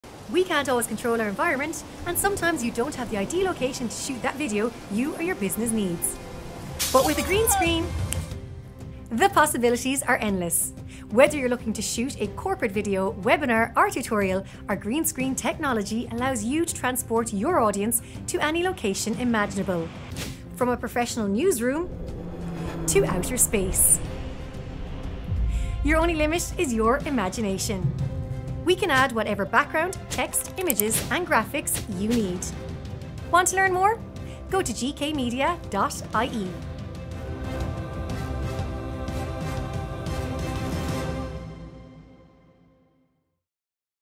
Green Screen Experience - Be Anywhere, Anytime!
With a green screen, the possibilities are endless.

Whether you're looking to shoot a corporate video, a music video, or even a scene in a film, our green screen technology allows you to transport your audience to any location imaginable.

This grants you complete control of your filming environment, avoiding unpredictable circumstances, simplifying your planning process and impressing the viewer with your unique visuals.